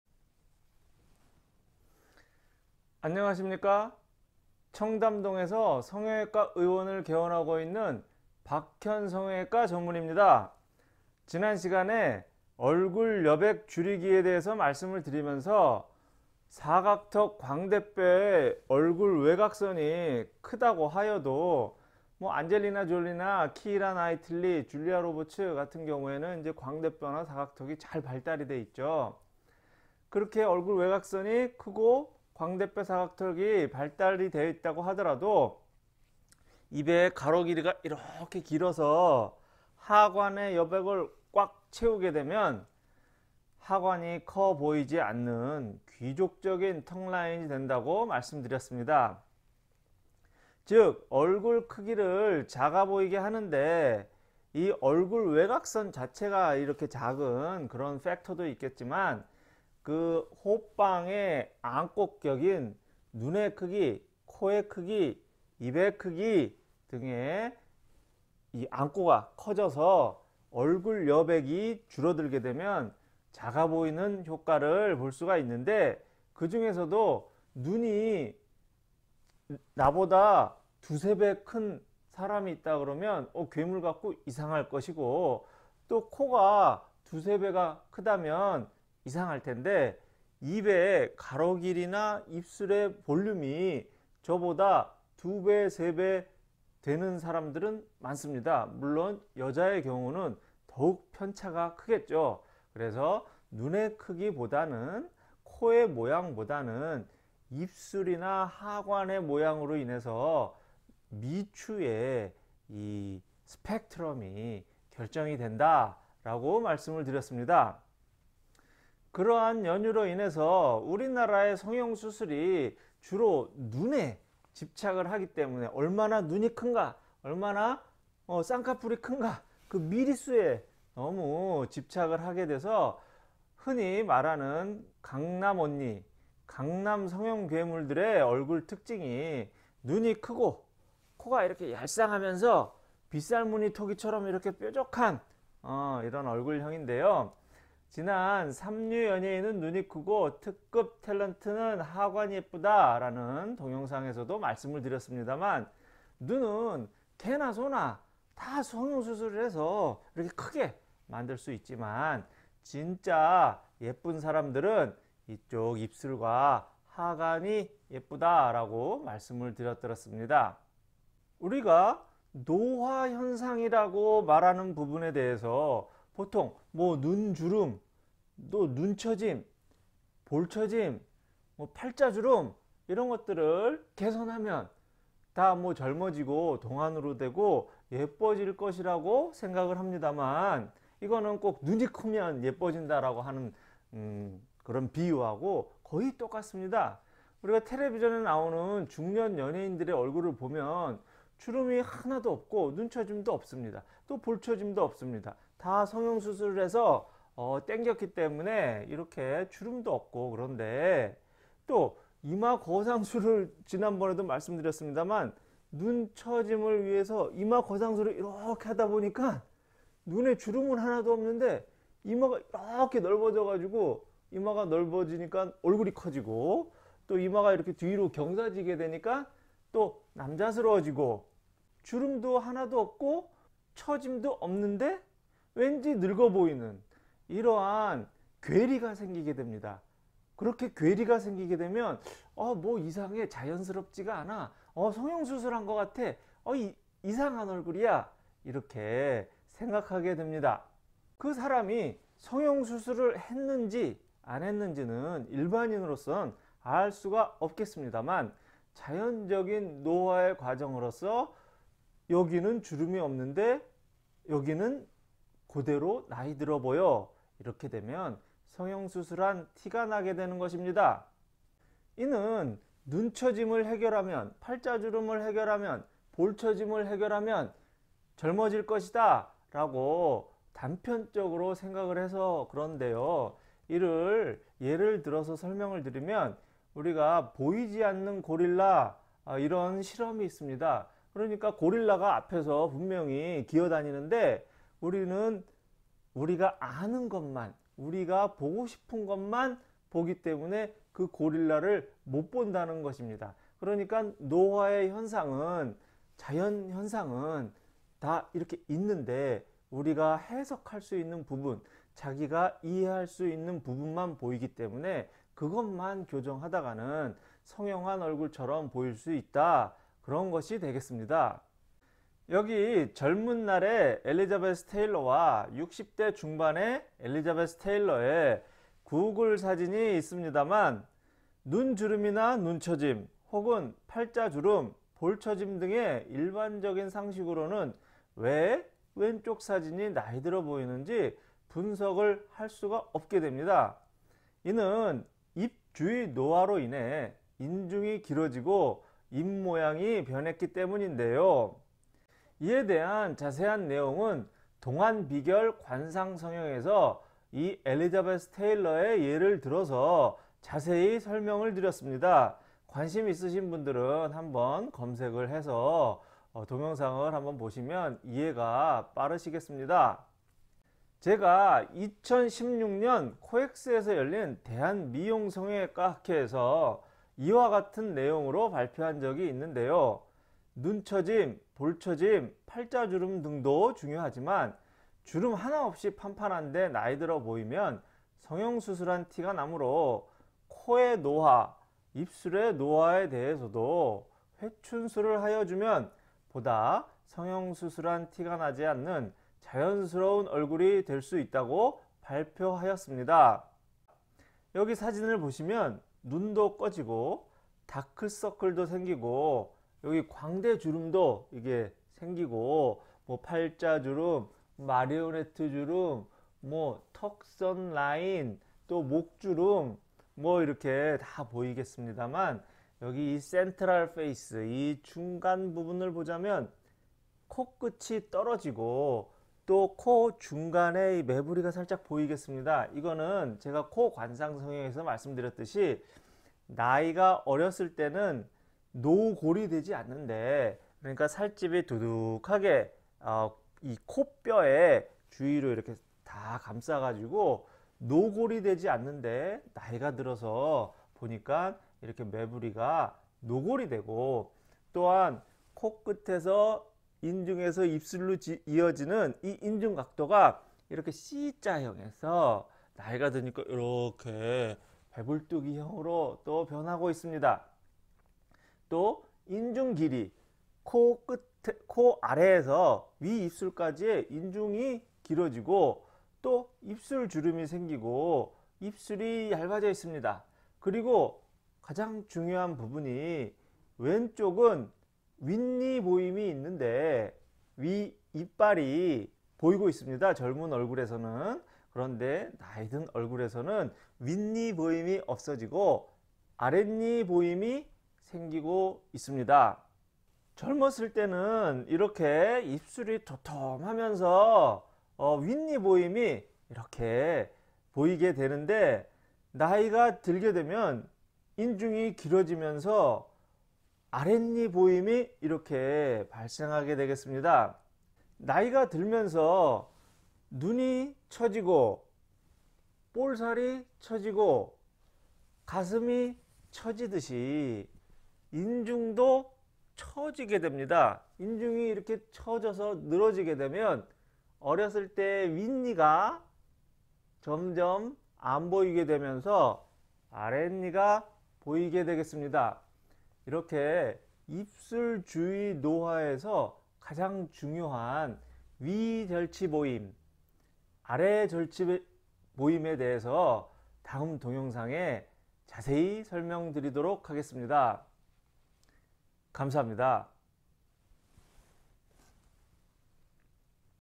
티안나게 예뻐지는 40대 50대 중년 미시 관상성형
지난시간에 얼굴 여백줄이기에 대해 말씀 드리면서
사각턱 광대뼈의 얼굴 외곽선이 크다고 하여도
(안젤리나 졸리 키이라 나이틀리 줄리아로버츠 등이 그러합니다만)
입의 가로 길이가 길~어서 하관 여백을 꽉 채우게 되면,
사각턱 광대뼈가 있으면서도 하관이 커 보이지 않는 귀족턱라인이 된다고 말씀 드렸습니다.

즉 얼굴크기를 작아 보이게 하는데
얼굴 외곽선 팩터도 있지만
앙꼬격인 눈의 크기 코의 넓이 입의 크기 등이 크면 얼굴 여백을 줄여서, 작아 보이게 할수 있는데,
그 중에서 입 크기의 편차가 가장 크다고도 말씀 드렸습니다.

우리나라의 성형이 눈을 크게 하는데에 집중이 되어 있어서 강남언니들의 얼굴 특징이 눈이 크고 코가 얇쌍하고 입이 작은 빗살무늬 얼굴형인데요
지난번 3류 연예인은 눈이 크고 특급 탤런트는 하관이 예쁘다~ 라는 동영상에서도 말씀 드렸습니다만
눈은 개나소나 다 쌍꺼풀 있고 큰 반면, 진짜 예쁜 사람은 입술과 하관이 예쁘다고 하였습니다.

노화 현상에 있어서도,
눈주름 눈처짐 볼처짐 팔자주름등을 주로 호소 합니다만
텔레비전에 나오는 중년 연예인들의 경우
눈처짐도 없고 볼처짐도 없는데
이마거상술해서 이마가 넓은
볼처짐은 없는데 왠지? 늙어 보이는.
‘보이지 않는 고릴라 (Invisible Gorilla) ‘처럼
아는것만 보이게 됩니다.

사람은 보고자 한 것만 보고, 듣고자 한 것만 듣는다
숲을 보지 못하고 나무만 본다

여기 젊은날의 엘리자베스 테일러와 60대 중반의 엘리자베스 테일러의 구글 사진이 있습니다만
눈 주름이나, 눈처짐, 혹은 팔자주름, 볼처짐등의 일반적인 상식으로는 왜? 왼쪽 사진이 나이 들어 보이는지? 분석을 할 수가 없게 됩니다.
이는 입 주위 노화로 인해 인중이 길어지고 입 모양이 변했기 때문인데요

이에 대한 자세한 내용은 동안비결 관상성형에서 이 엘리자베스 테일러의 예를 들어서 자세히 설명 드렸습니다. 관심있으신 분들은 한번 검색해서 보시길 바라고요

이에 대해서는 2016년 코엑스에서 열린 대한 미용성형외과 학회에서 이와 같은 내용으로 발표한 적이 있는데요
눈처짐, 볼처짐 팔자주름 등도 중요하지만
주름하나 없이 판판한데 나이 들어보이면 성형수술한 티가 나므로
코의 노화 입술의 노화에 대해서도 회춘술을 하여 주면 보다 성형수술한 티가 나지 않는 자연스러운 얼굴이 될수 있다고 발표 하였드랬습니다.

2003 지방이식 대가 시드니 콜맨
이와 같은 아름다운 삽화
눈도꺼지고, 다크써클도 생기고 광대주름도 생기고, 팔자주름 턱선과 마리오네트 주름 목주름
central face 를 보자면 코끝이 떨어지고 노골이 되고 인중각도가 C자 형에서 배불뚝이 형으로 변하고 인중이 길어지고 입술주름이 생기고 입술이 얇아지고,,,
그리고 왼쪽은 윗니보임이 있는데 오른쪽은 아랫니 보임이 있게 됩니다.

즉 젊었을때는
윗니보임이 이렇게 입술이 얇던 두껍던 간에
이렇게 인중이 길어지고 입술이 아랫니 보입이 발생하게 되겠습니다.
그럼 다음 동영상에는 윗니보임 윗절치보임, 아랫니보임, 아랫절치 보임에 대해서 자세히 설명 드리도록 하겠습니다.